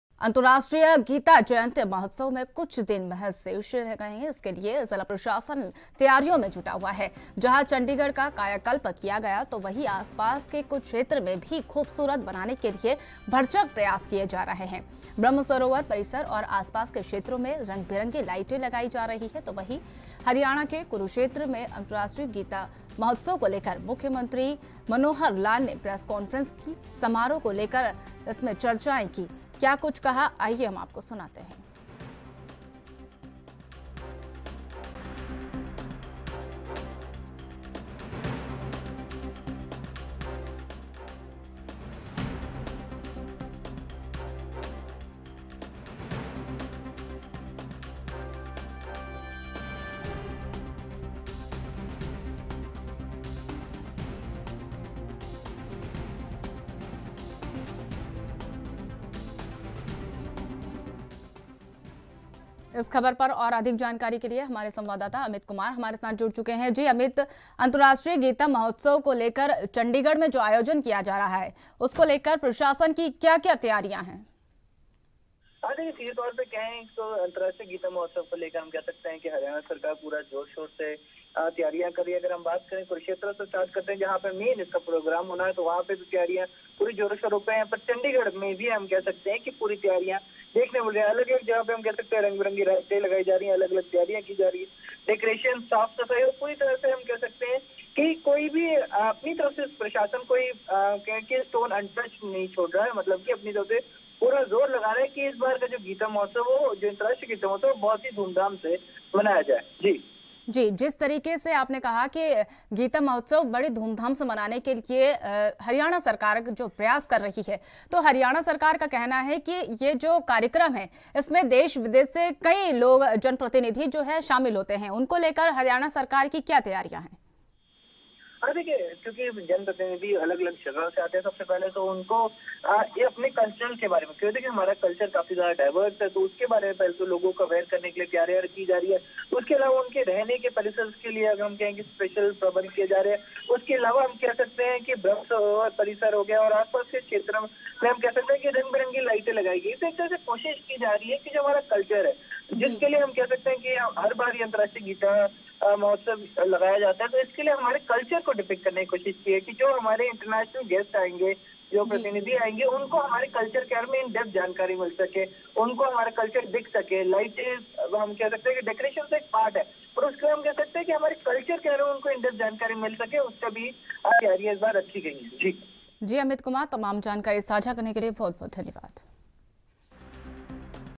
सीएम मनोहर लाल ने की प्रेस कॉन्फ्रेंस, अंतरराष्ट्रीय गीता महोत्सव पर की चर्चा | ANB NEWS
हरियाणा के सीएम मनोहर पहुंचे कुरुक्षेत्र
अंतरराष्ट्रीय गीता महोत्सव का आयोजन
सीएम मनोहर लाल ने की प्रेस कॉन्फ्रेंस
अंतरराष्ट्रीय गीता महोत्सव पर की चर्चा